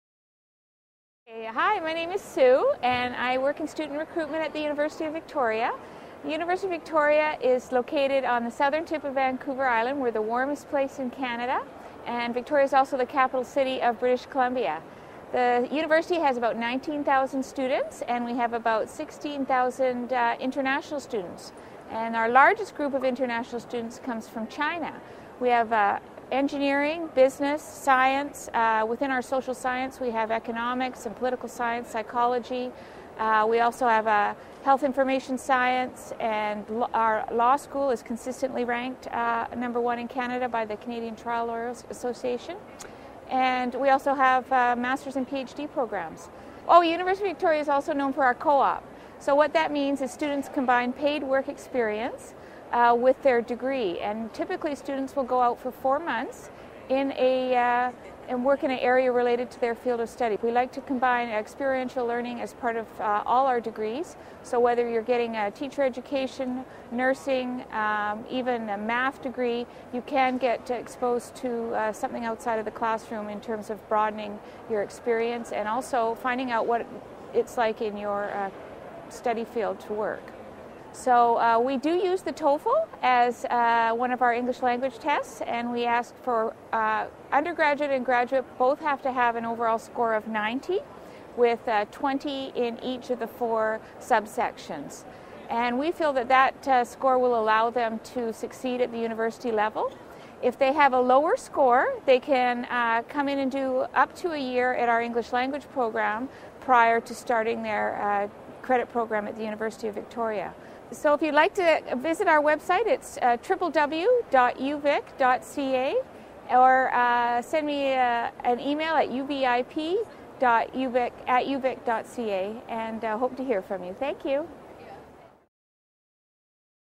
TOEFL® Destinations: University of Victoria (Canada)
Sue Butler, International Student Recruitment Coordinator at the University of Victoria, speaks about the university's unique co-op program and the TOEFL® test.

Note: YouTube is banned/blocked in China, therefore links to Chinese social properties have been excluded.